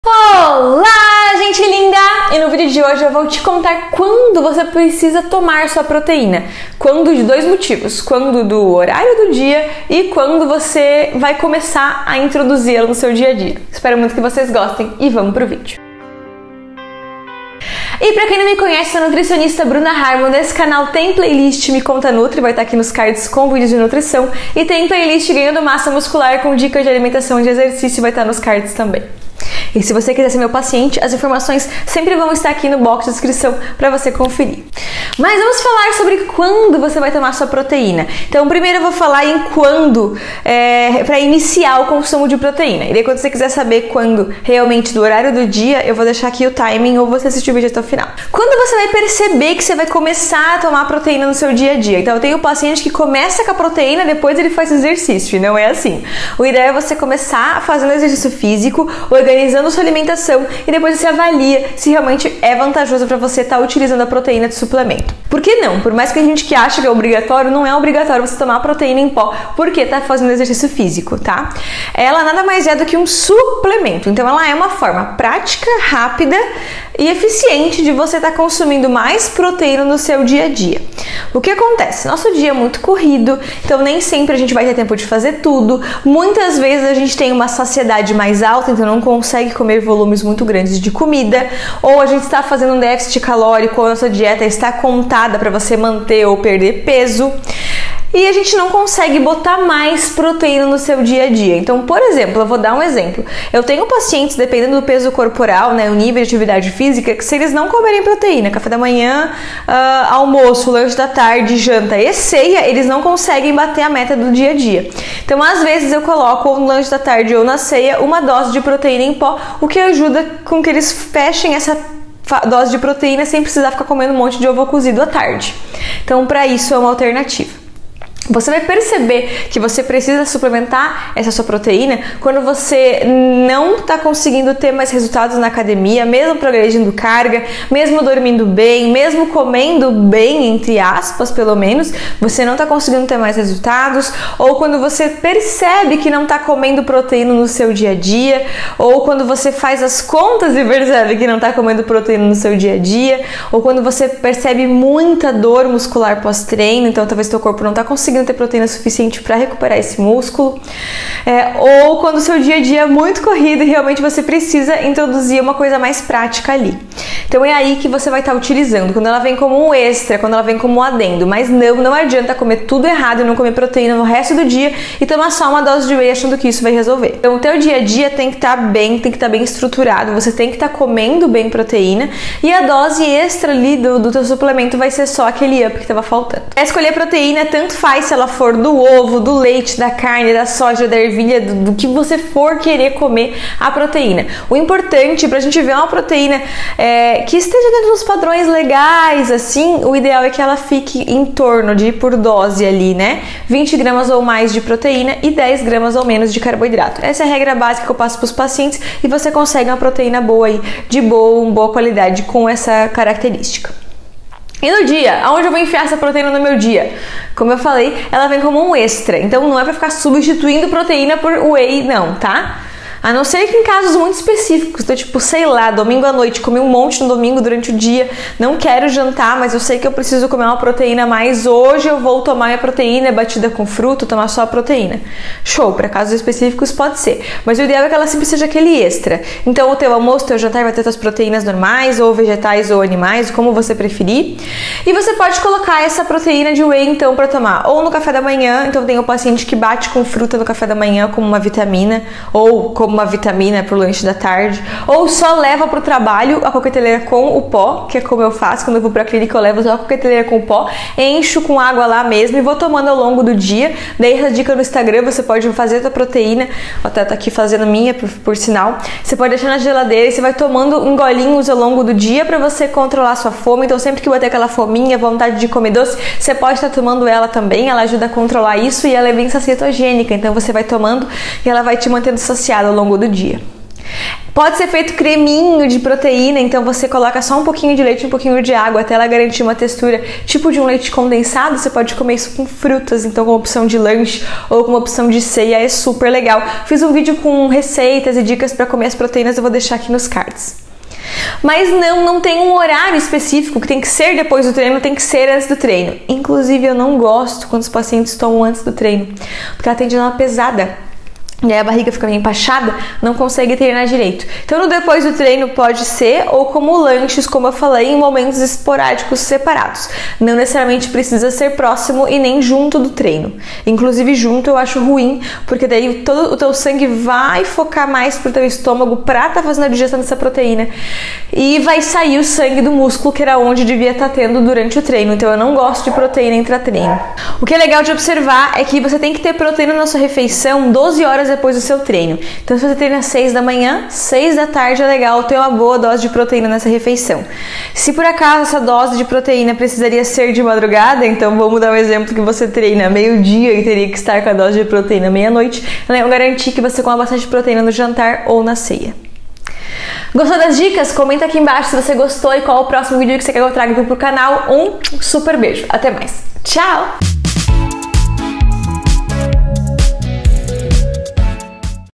QUANDO COMEÇAR A TOMAR PROTEÍNA - BRUNA HARMEL
E no video de hoje eu te contei quando começar a tomar proteína, , espero que gostem.

Bruna Harmel
CAIXA POSTAL 03
Barra Velha - SC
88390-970